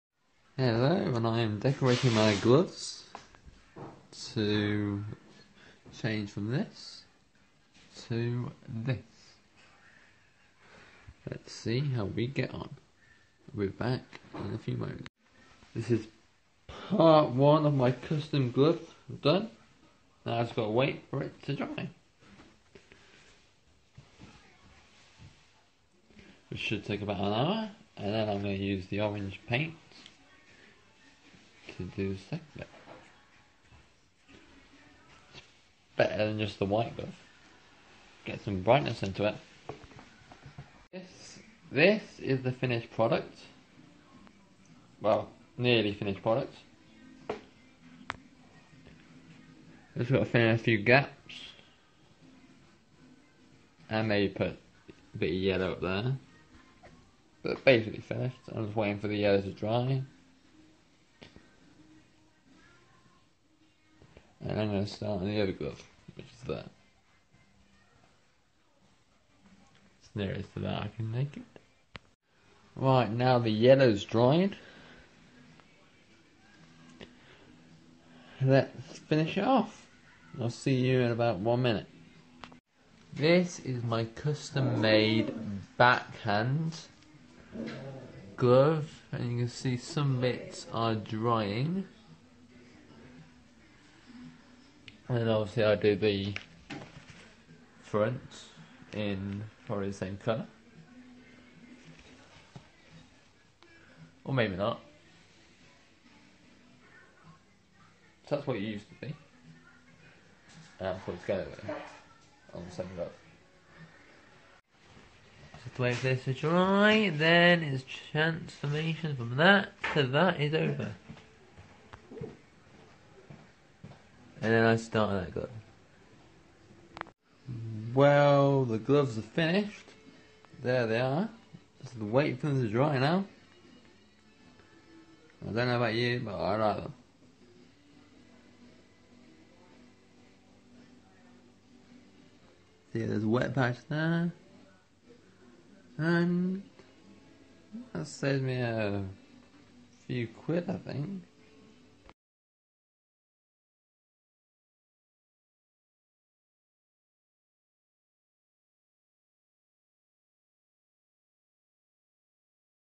Custom made Nike gloves
Here is me custom makeing my gloves to a different color to put soom color in the gloves